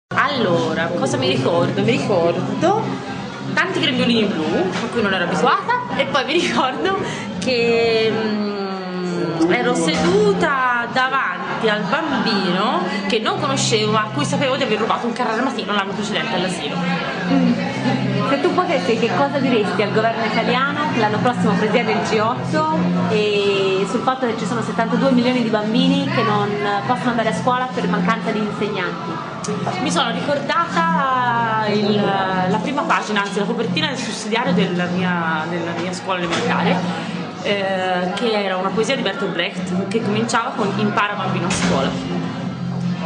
Istruzione per tutti Simona ricorda il suo primo giorno di scuola
Ricordi il tuo primo giorno di scuola? Che cosa hai provato quel giorno? Chi hai incontrato? Certamente ha cambiato la tua vita... 74 milioni di bambini non hanno accesso ad un'istruzione. Se i leader G8 non mantengono le promesse fatte in passato, probabilmente non proveranno mai l'emozione del primo giorno di scuola...